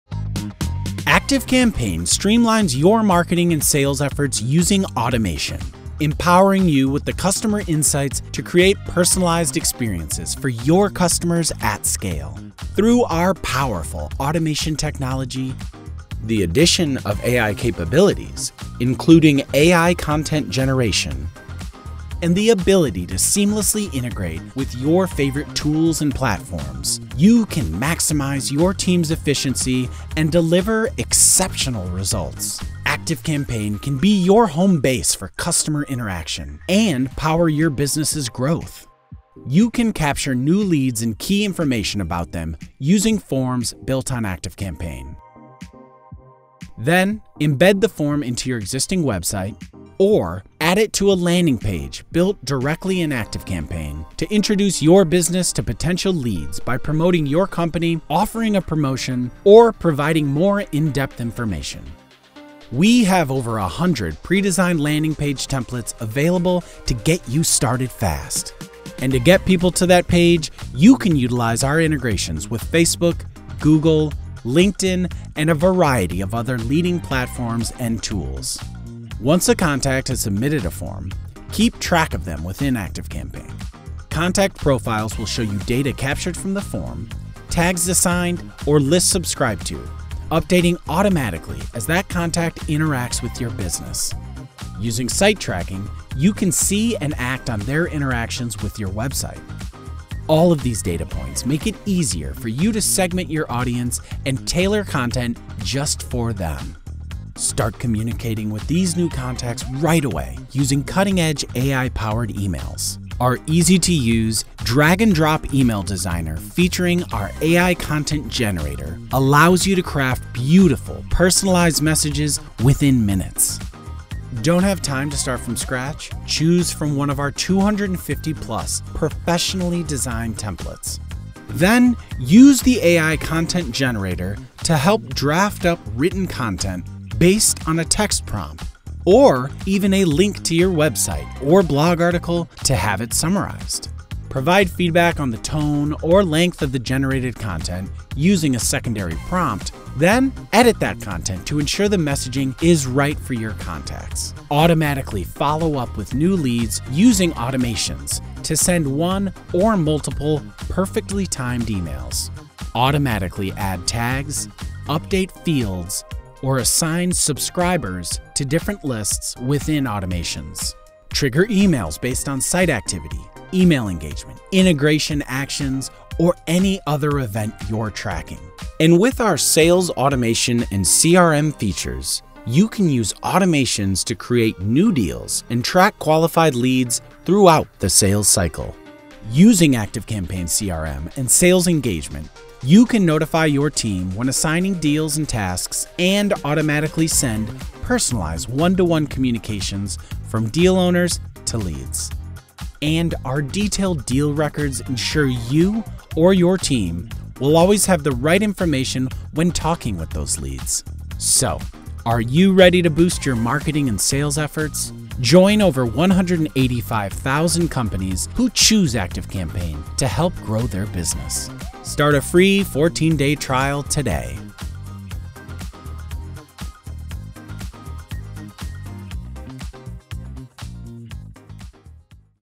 ActiveCampaign platform demo in 4 Minutes!
In this 4 minute platform demo, we show you how you can transform your marketing and sales with ActiveCampaign!

Discover how our powerful automation and AI capabilities streamline your efforts, giving you deep customer insights to create personalized experiences at scale. With dynamic forms and landing pages supported by hundreds of pre-designed templates, seamless integrations, and AI assistance,  you can capture leads effortlessly and promote your business effectively.

Our intelligent email system simplifies email creation with drag-and-drop design and AI content generation. Tailor messages to your audience, and automate follow-ups with perfectly timed emails. Plus, our sales automation and CRM features can help you track leads and deals, ensuring your team has the right information to close deals effectively.

Join the 185,000+ businesses already using ActiveCampaign to supercharge their growth. Start your free 14-day trial today and take control of your marketing and sales success:

00:00 Introduction
00:39 Capture leads using forms and landing pages
01:10 Use integrations to drive people to your site
01:24 Contact profiles, site tracking, and segmentation
01:48 Email Designer, AI content generator, and campaigns
02:35 Automations - automatically email and followup with contacts and leads
02:58 Sales Engagement Automation and CRM